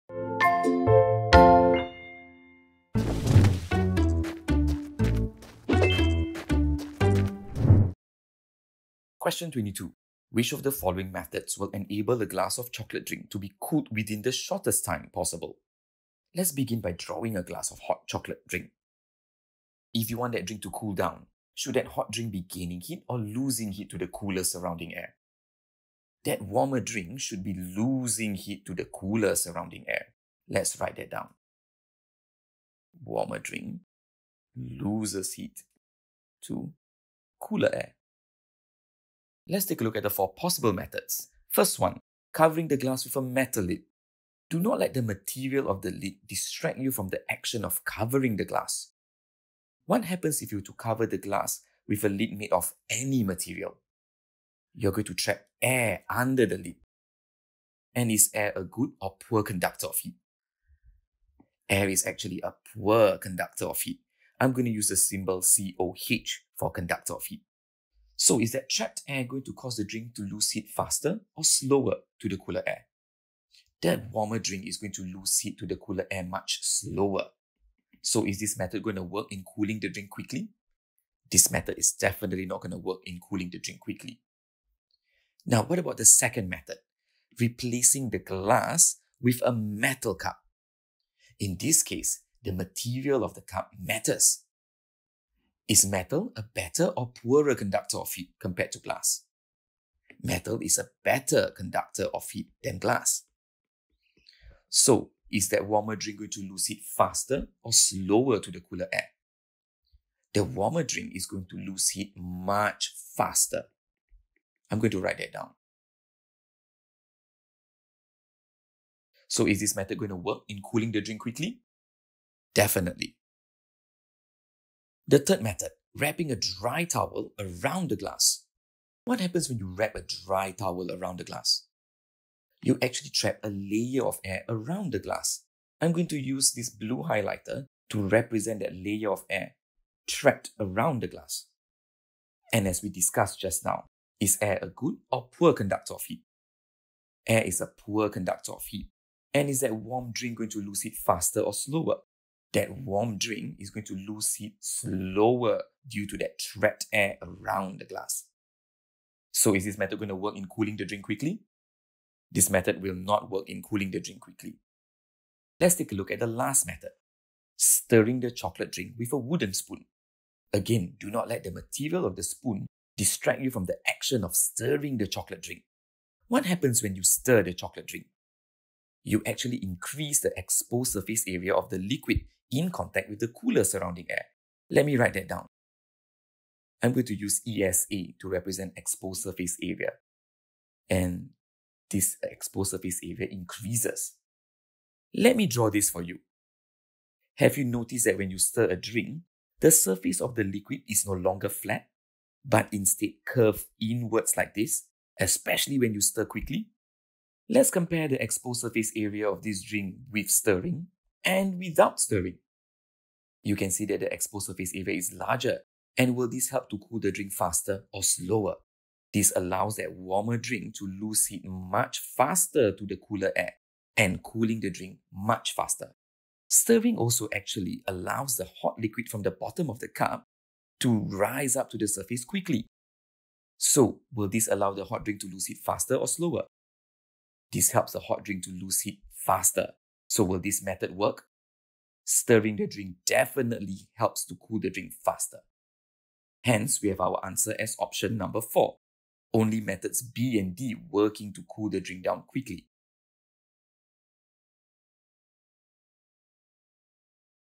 EXAMINATION PAPER SERIES | 2019 SNGS P5 SA2 | Question 22 | Heat Energy 🔥
In this Examination Paper Series, Mr. Firhad will be analysing Question 22 from the 2019 CHIJ St. Nicholas Girls' School (SNGS) P5 SA2 Science Examination paper.

Watch closely as he shares his approach in tackling this question on Heat Energy!